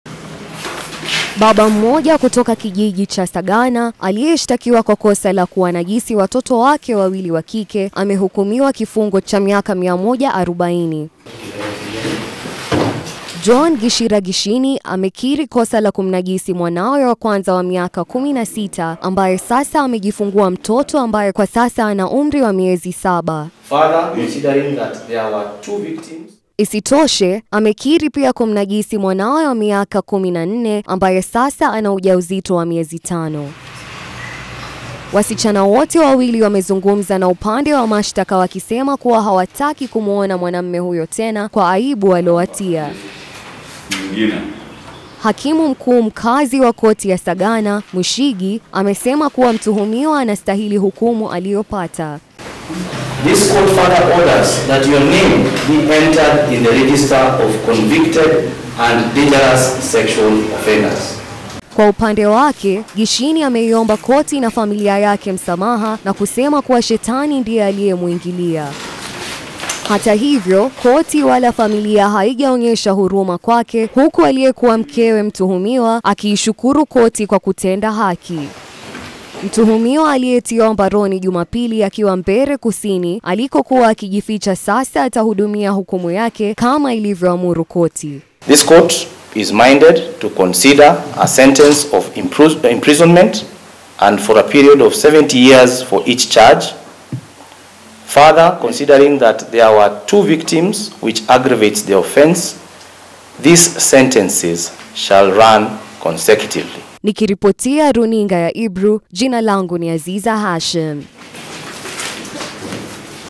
Mahakama Yamfunga Baba Aliyepachika Watoto Mimba
Mhubiri Mmoja Kutoka Eneo La Kirinyagaaliyekiri Kuwanajisi Watoto Wake Na Kisha Kuwapachika Mimba Amefungwa Kifungo Cha Miaka 140 Jela. John Gichira Mhubiri Wa Kanisa La Akorino Atahudumu Miaka Sabini Kwa Kila Kosa Kwa Mujibu Wa Hakimu Mkuu  Wa Baricho Antony Mwicigi.